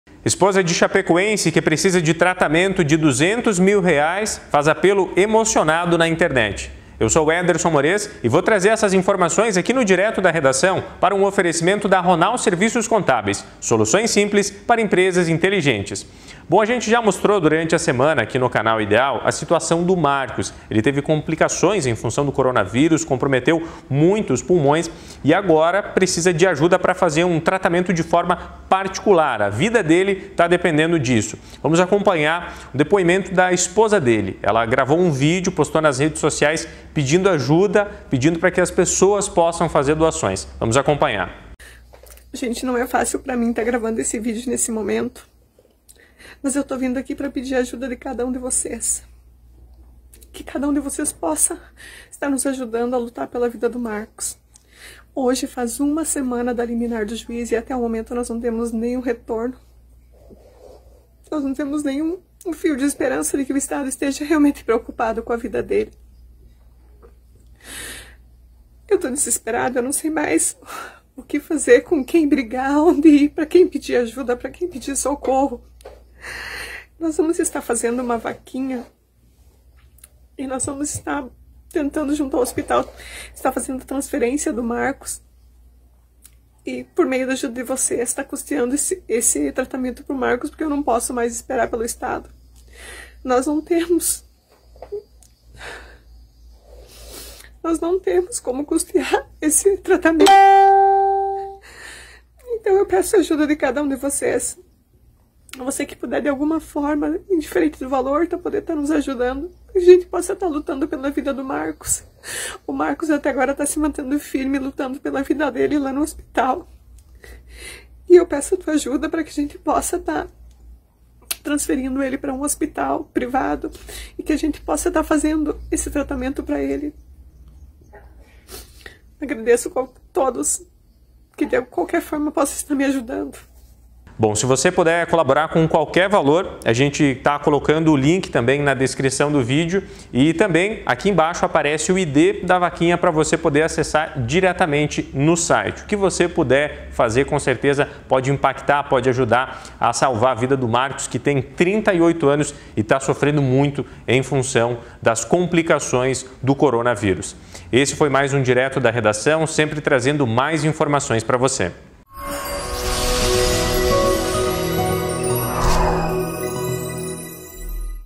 Esposa de Marcos Drumm faz pedido de ajuda emocionado
O morador de Chapecó, precisa de um tratamento de R$ 200 mil e a família está pedindo doações. Para contribuir acesse